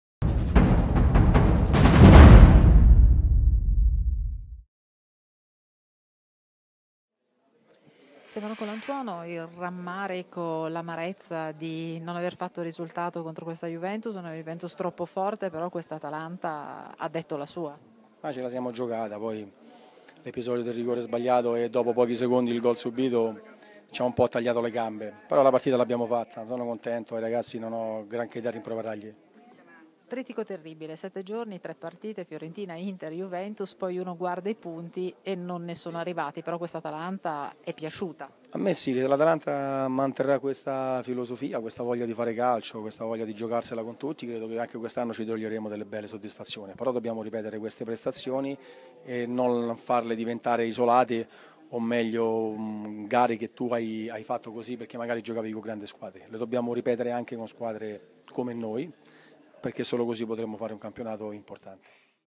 Stefano Colantuono sulla partita Atalanta-Juventus
Il tecnico nerazzurro commenta la prestazione della suasquadra in campo contro la Juventus campione d'Italia.